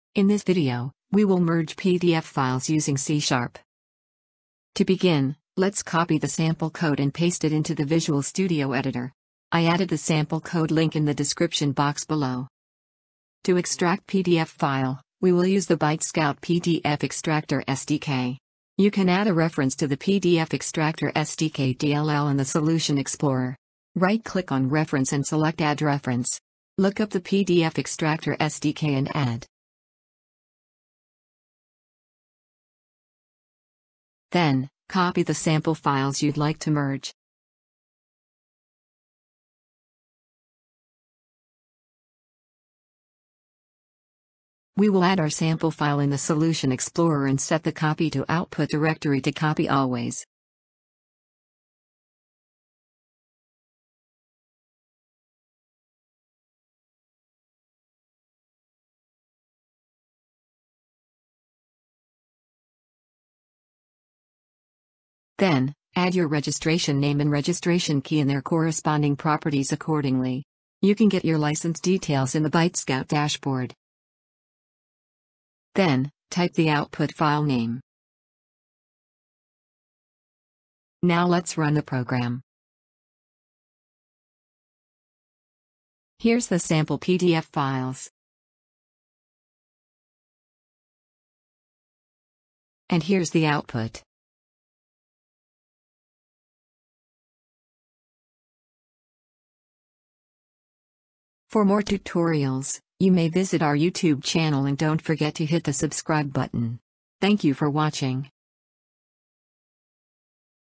ByteScout PDF Extractor SDK - Merge PDF Files
In this video, you're going to learn how to merge PDF files using the C# programming language. You may check the article for this tutorial